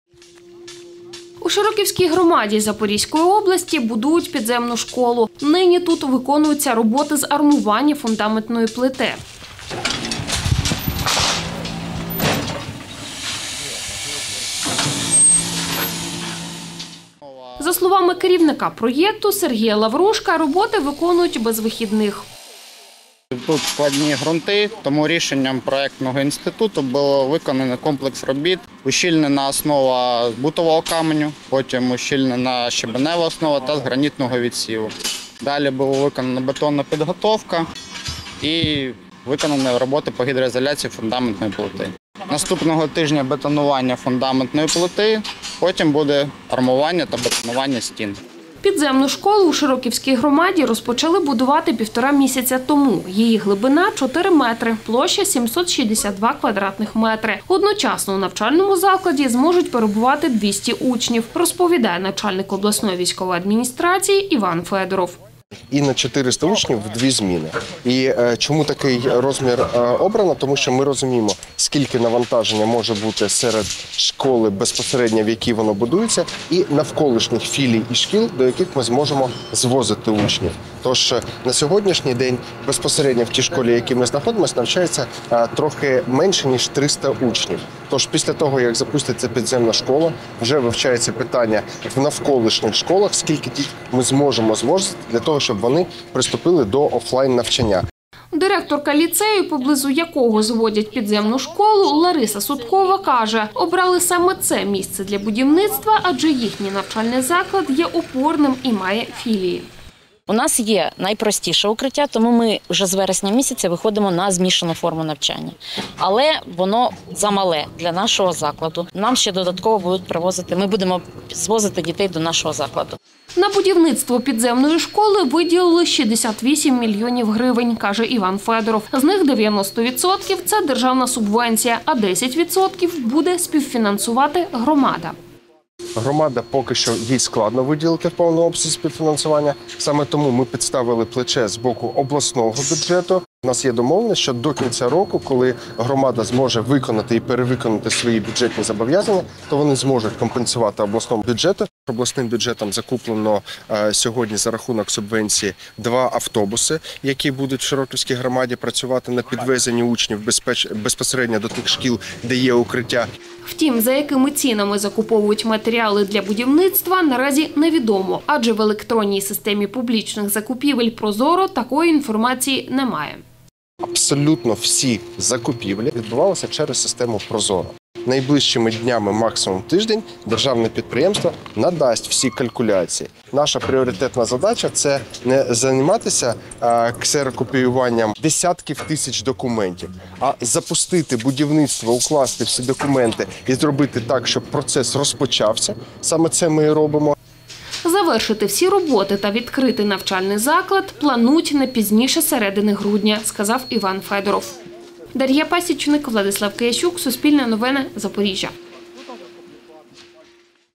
У Широківській громаді Запорізької області будують підземну школу
Підземну школу будують у Широківській громаді Запорізької області. Майбутній навчальний заклад буде розташований на площі 762 кв.м та на глибині — 4 метри. За словами начальника обласної військової адміністрації Івана Федорова, тут одночасно зможуть навчатися 200 учнів. Коли завершать будівництво та яка його вартість — детальніше у сюжеті Суспільного.